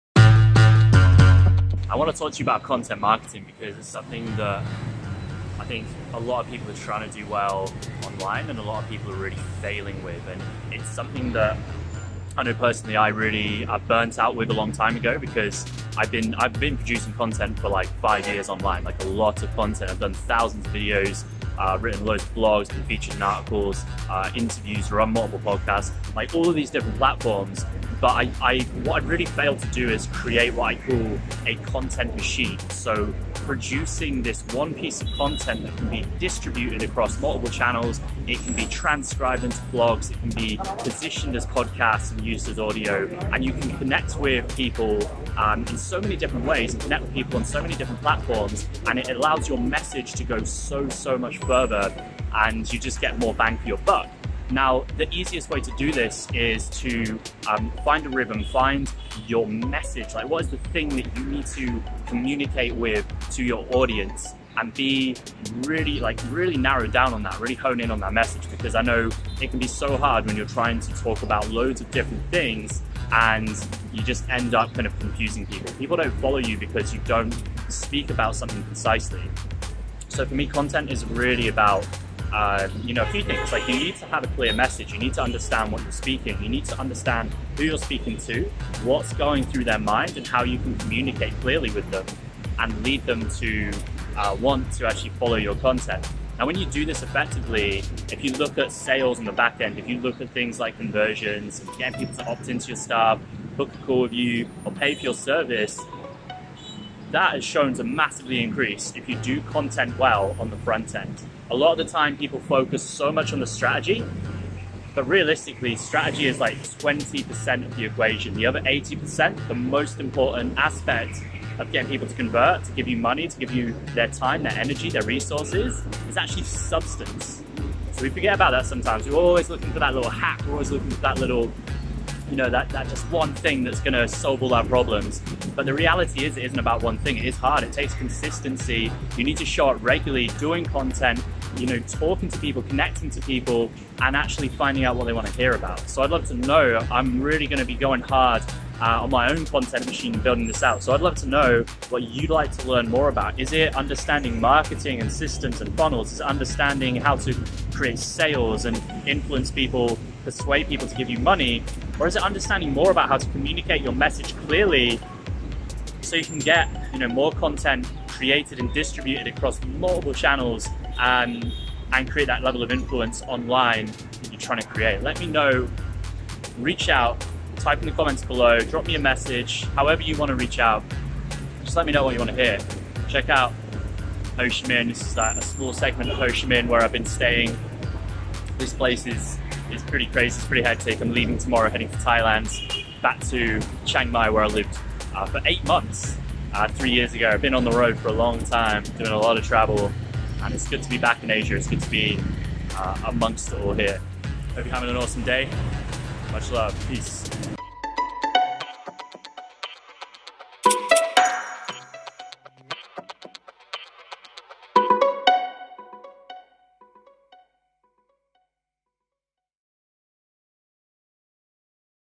THE BIGGEST MISTAKE PEOPLE MAKE WITH CONTENT MARKETING
A lot of people with high-ticket offers will be familiar with content marketing.
Using organic content to create an audience to sell to.
However, most people fall short when it comes to the most important factor.